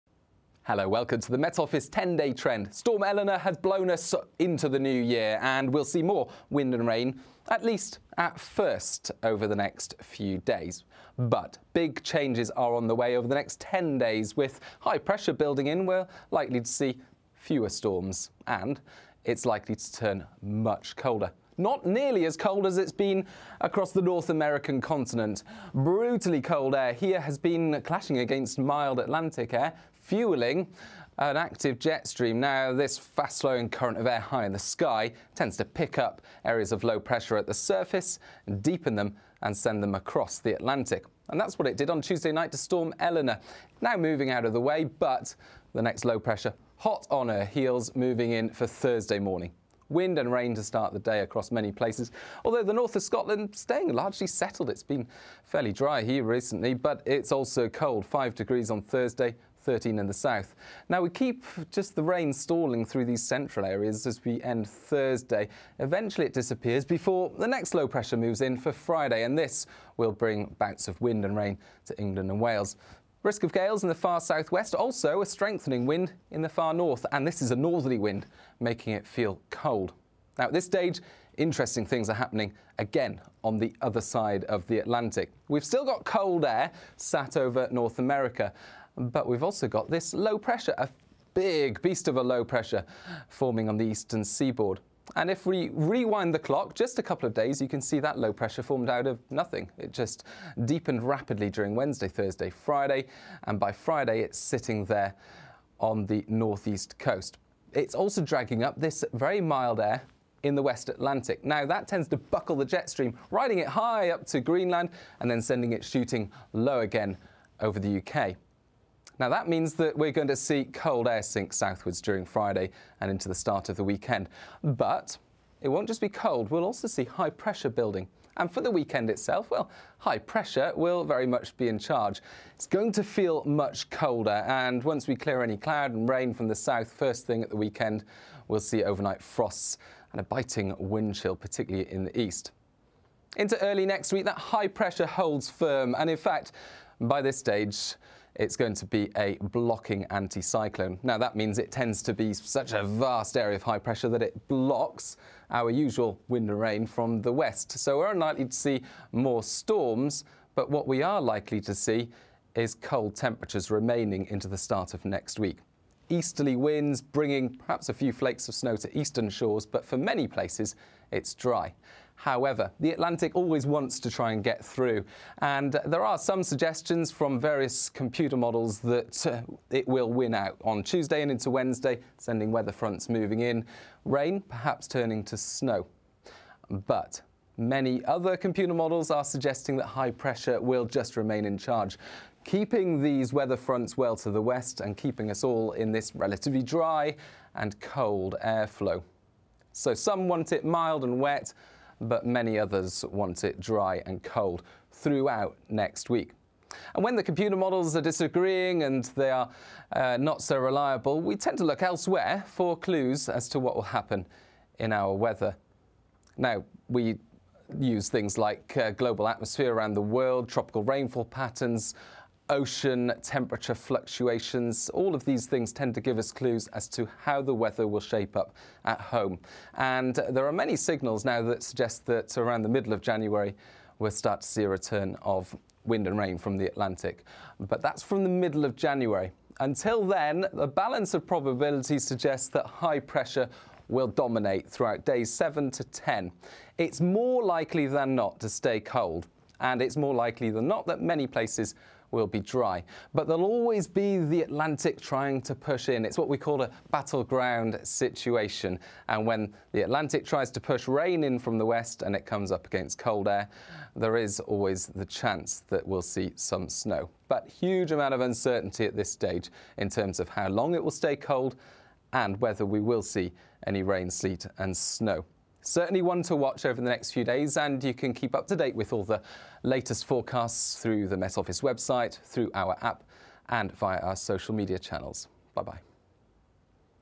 10 Day Trend - Following Storm Eleanor, winter bites back
Aidan McGivern takes a look at the weather for the next 10 days.